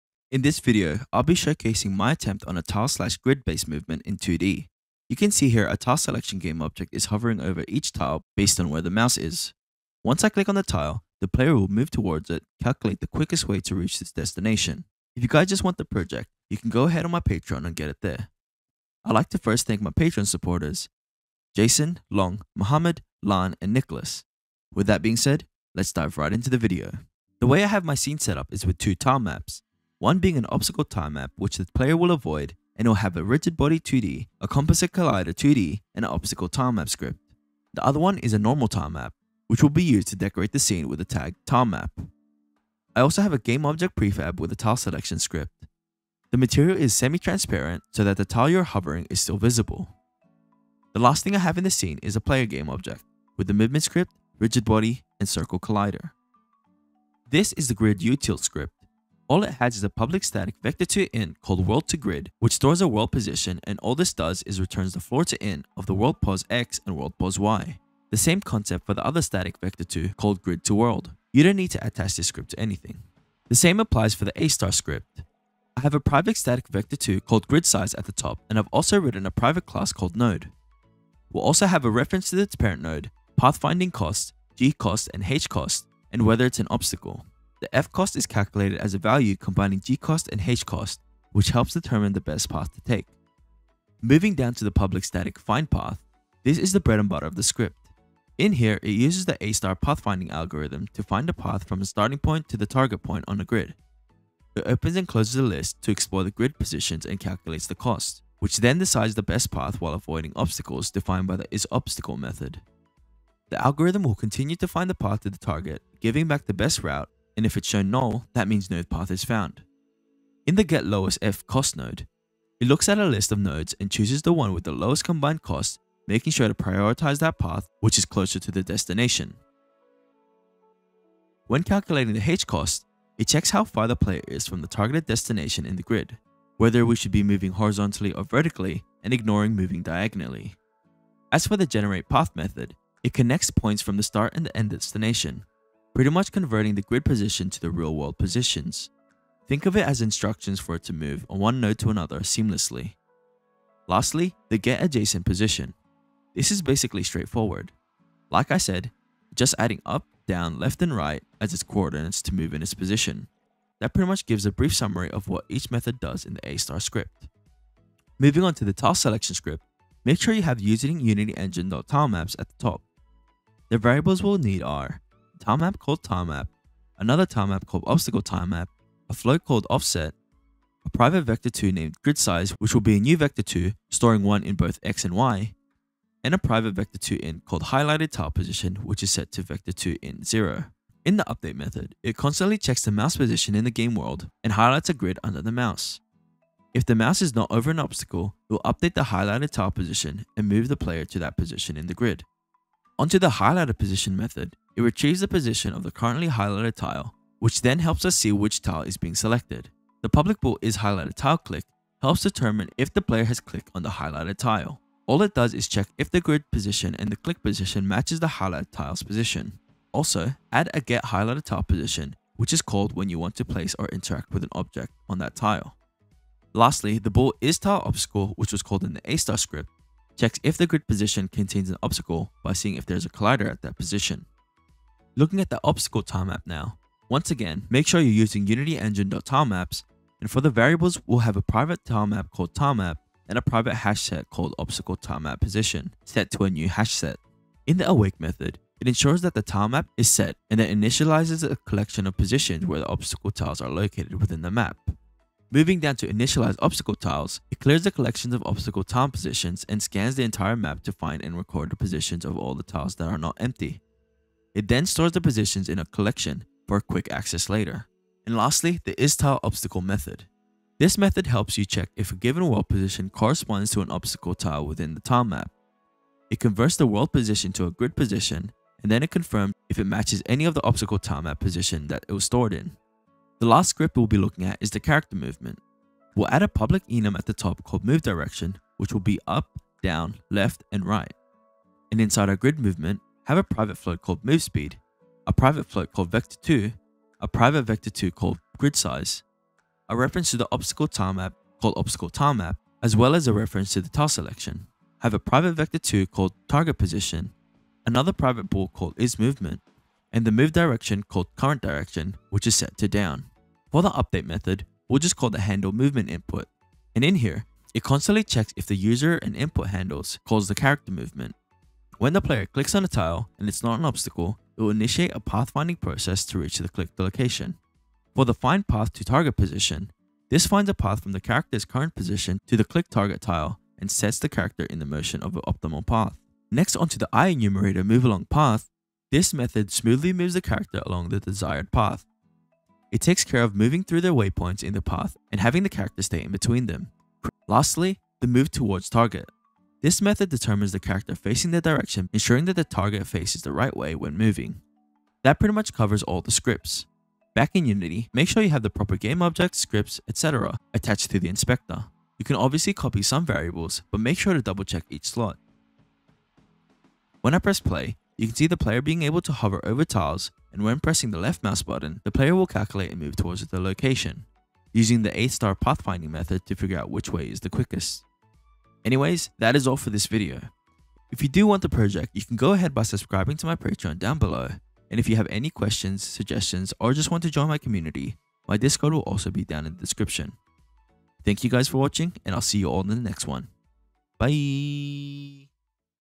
Unity 2024 Tutorial: SIMPLE Grid-Based CLICK Movement [Part 1]
Showcasing my attempt on a tile/grid based movement in 2D. Hovering over each tile based on where my mouse is. Once I click on that tile, the player will move towards it, calculating the quickest way to reach its destination.

Discord DM: VinnyDev6490

0:00 - Intro
0:27 - Set Up Scene
0:56 - Grid Utils Script
1:13 - A Star Script
2:47 - Tile Selection Script
3:54 - Obstacle Tilemap Script
4:42 - Movement Script
5:57: Results & Outro